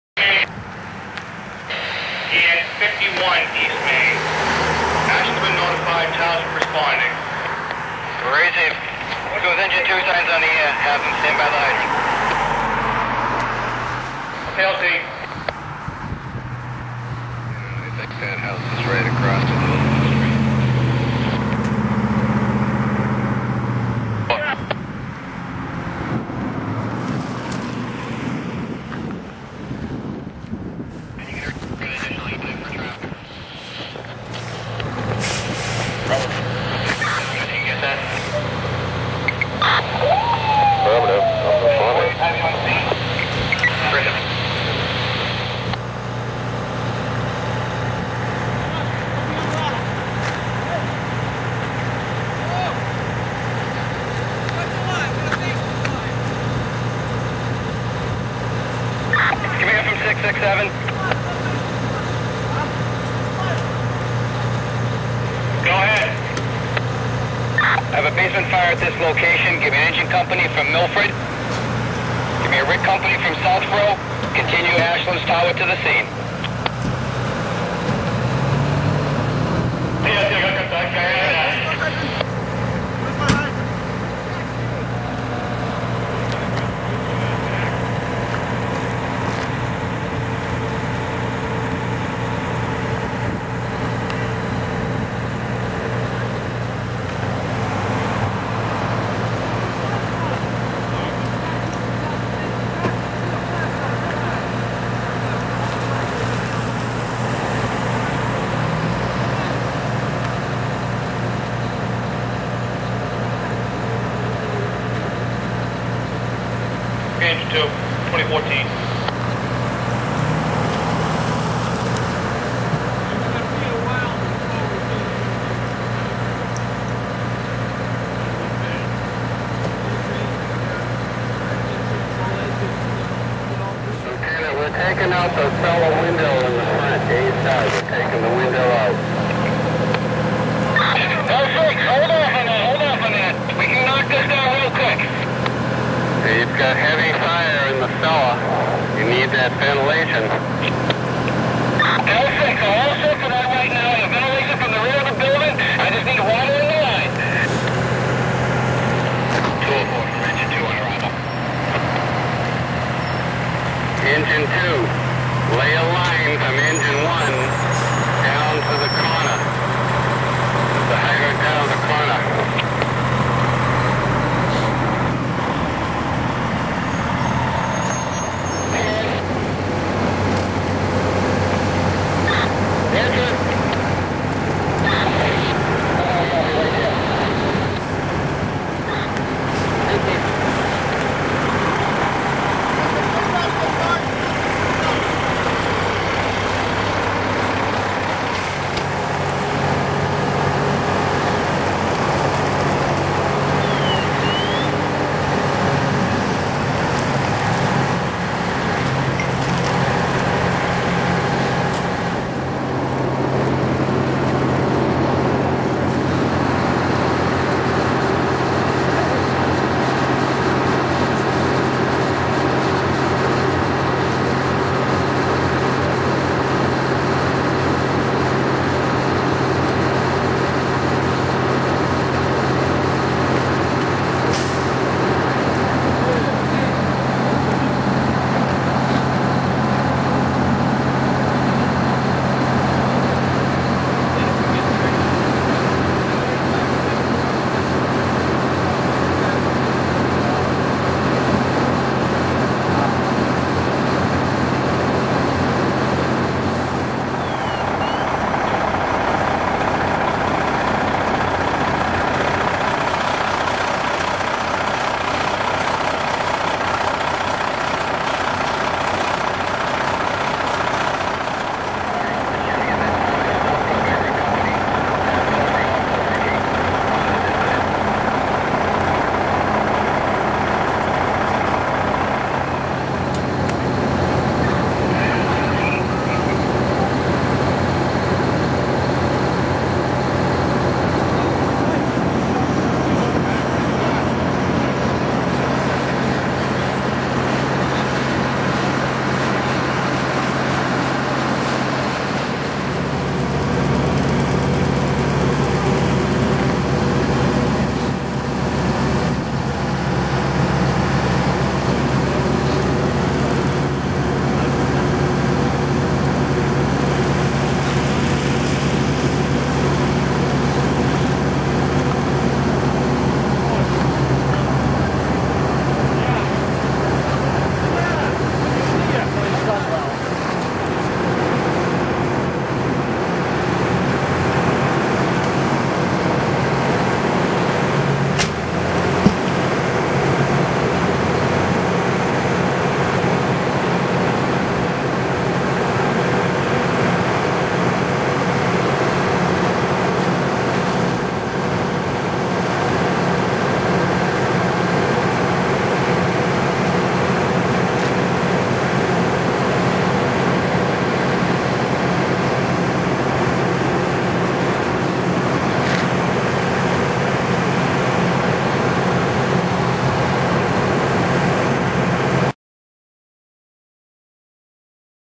HopNews - Basement Fire on East Main Street
Hopkinton Fire Department responds to a Basement Fire on East Main Street.